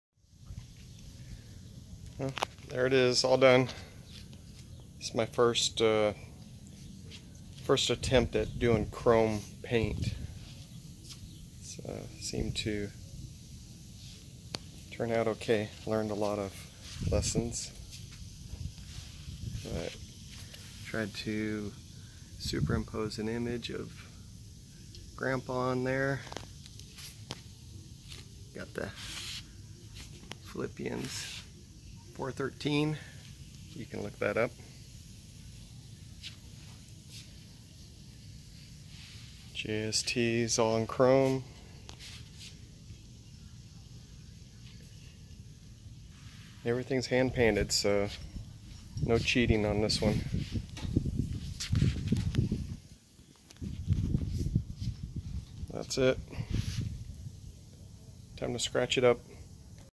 Jake's New Carbon Fiber Custom Chrome Painted Simpson Helmet
Jake's New Carbon Fiber Custom Chrome Painted Simpson Helmet. This was my first attempt at doing the chrome paint with the House of Kolor color tints. It was fun to do and I learned a lot.